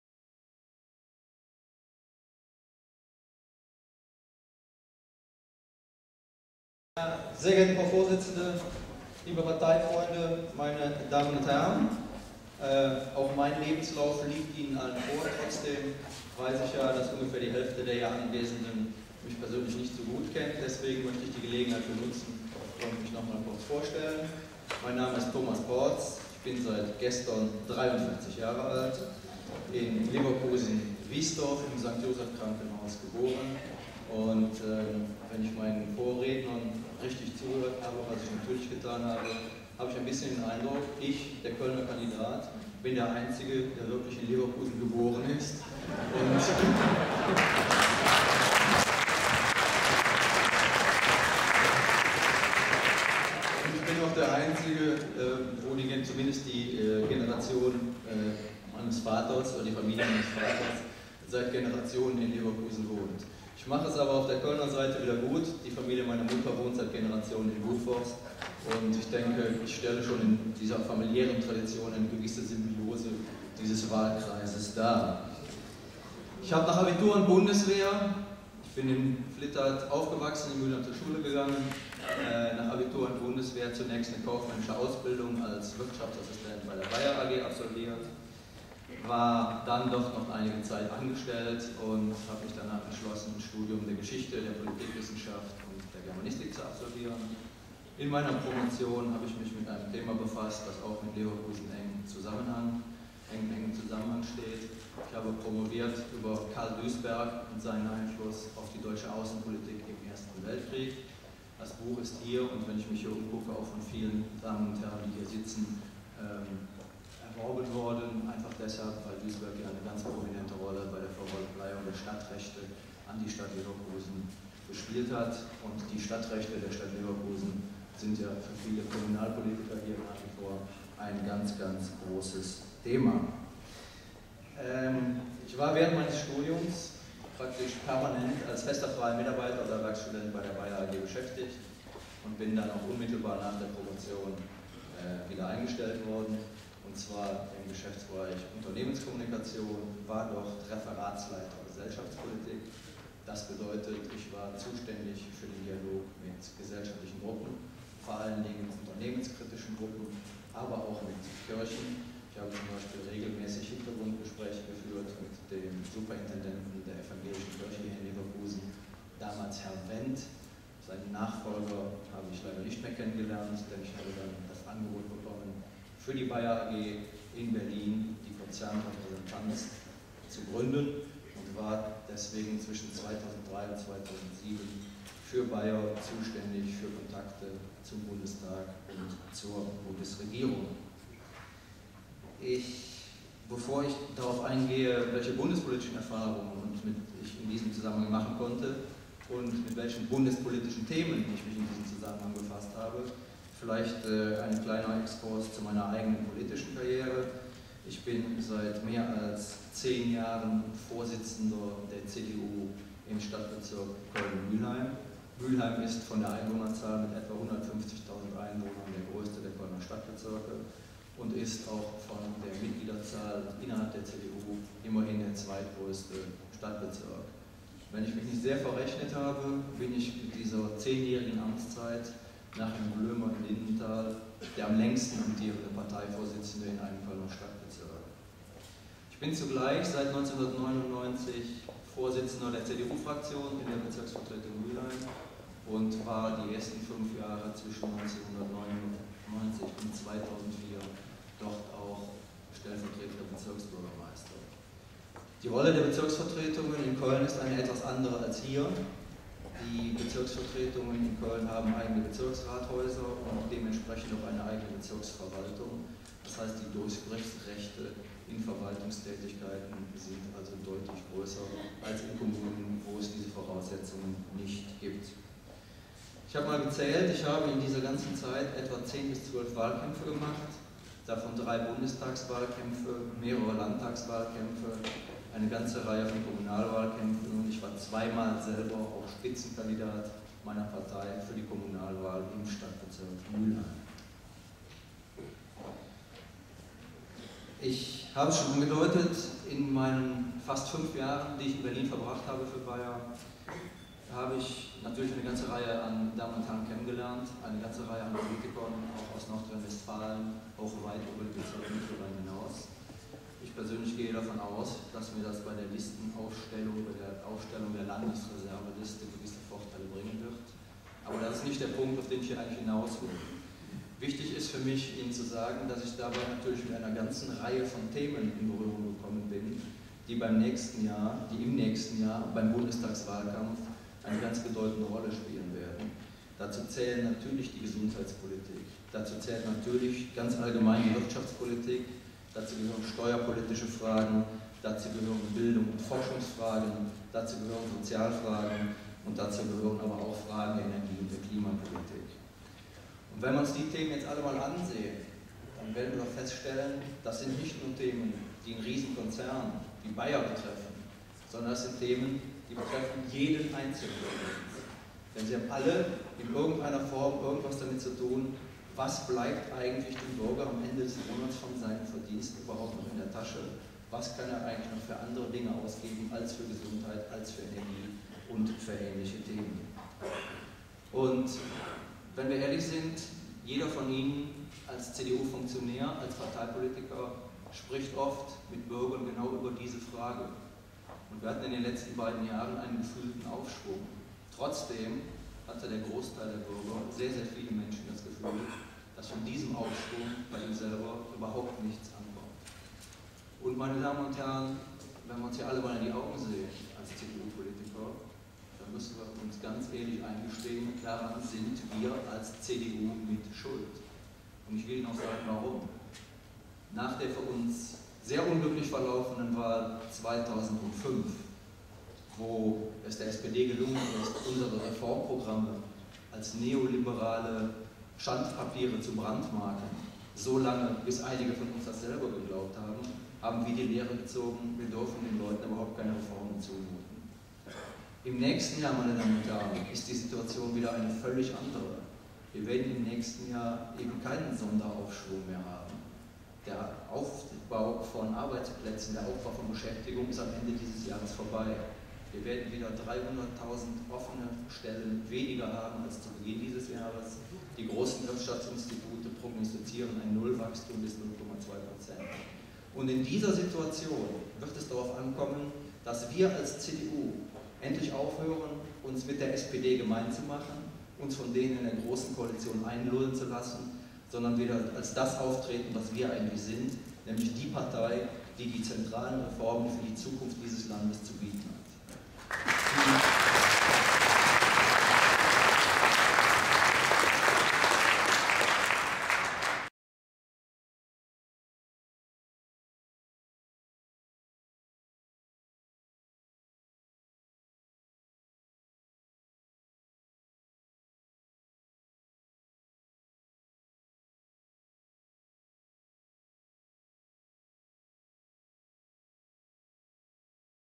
Wahl CDU-Kandidat für Bundestagswahlkreis 102, Teil 9a (27.10.2008)
Aufgenommen im Leverkusener Forum